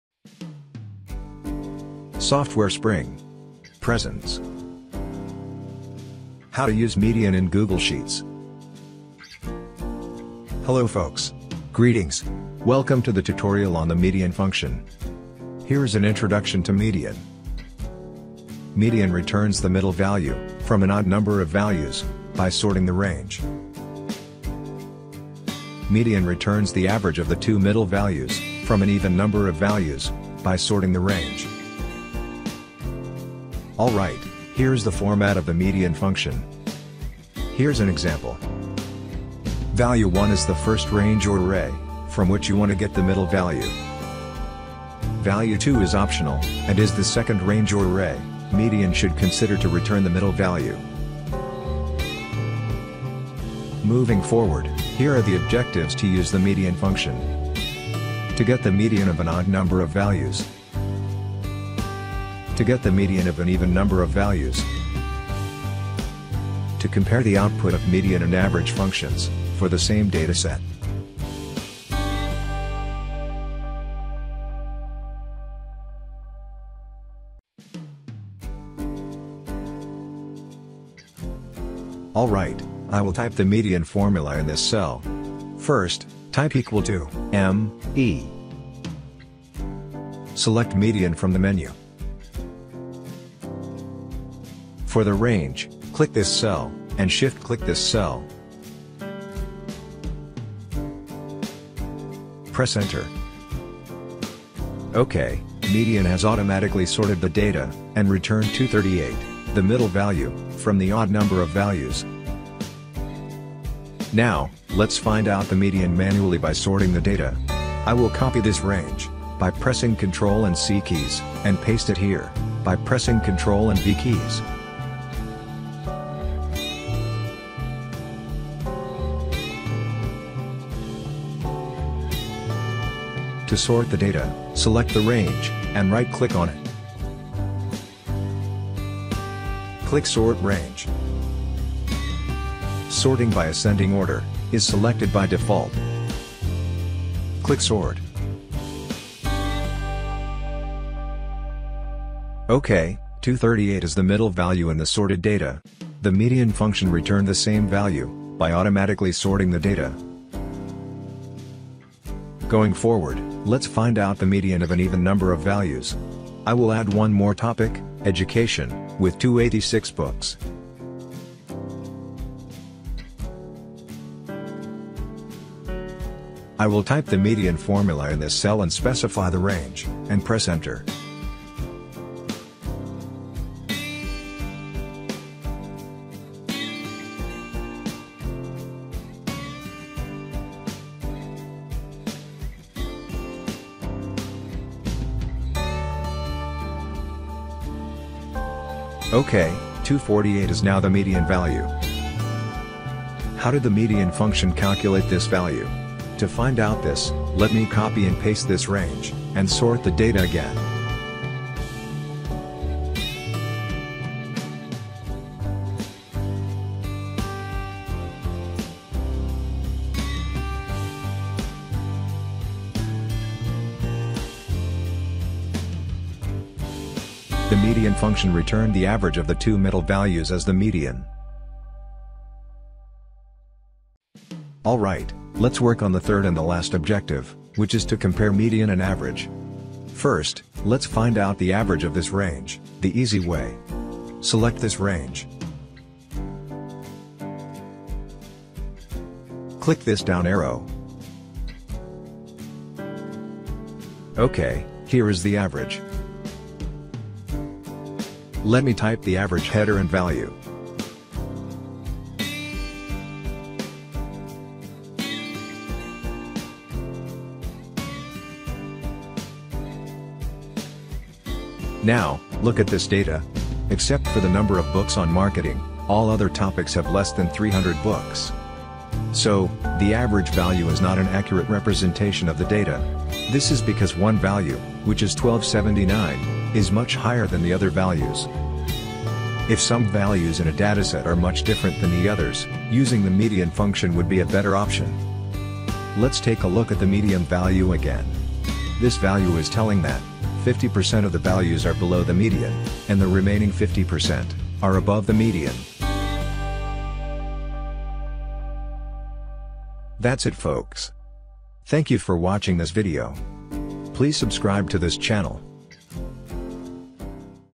Google Sheets MEDIAN Function | Get the Middle Value from a Dataset | How to Use MEDIAN Function
Use the Google Sheets MEDIAN function to get the middle value in a dataset, which is sorted in an ascending order. The MEDIAN function automatically sorts the data to return the median value. It means you do not have to sort the data before using the MEDIAN function in Google Sheets.

If a dataset has an odd number of values, the middle value is the median. In case of even number of values, the MEDIAN returns the average of the middle two values. The middle two values are those that have an equal number of values before the first and after the second middle value.

The MEDIAN function, along with AVERAGE and MODE, is typically used in statistical analysis of data.

If you are interested to know more about the MODE and MODE.MULT functions, here are the links to their video tutorials:

You may want to use the MEDIAN function instead of the AVERAGE, if some of the values in the dataset are much different than the rest of the values.

Let's look at the format of the Google Sheets MEDIAN function:

=MEDIAN(value1, [value2], ...)

MEDIAN is the name of the function.

value1 is the first value or range, MEDIAN should consider to return the middle value.

value2 is optional, and is the second value or range, MEDIAN should consider.

Here is an example of the MEDIAN function:

Assume that cells B1 to B5 has the salaries, in USD, of five employees. Let B1=2922, B2=2665, B3=2876, B4=2569, and B5=4301.

If you calculate the average salary, it will be 3066.60. From the average, we can infer that it's not an accurate representation of the salaries, because one salary value, 4301, is causing data distortion. Note that except for this salary, all others salaries are below 3000.

So, let's use the same data and calculate the median salary

=MEDIAN(B1:B5)

=2876

Now, the above value is an accurate representation of the salary data.

Although the data in the range B1 to B5 is unsorted, as said before, the MEDIAN function automatically sorts the data and calculates the median.

Do take a look at this video tutorial, which gives the steps to use the Google Sheets MEDIAN function with examples.